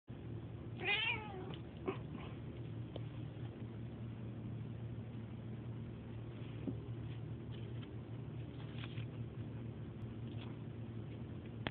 Messing with Jackass
I put a leaf in my cats collar to see how she reacted. I had a funnier version but i accendently deleted it lol. And yes my cats real name is Jackass. I used to call her Stinky when i wasnt comfortable saying Jackass.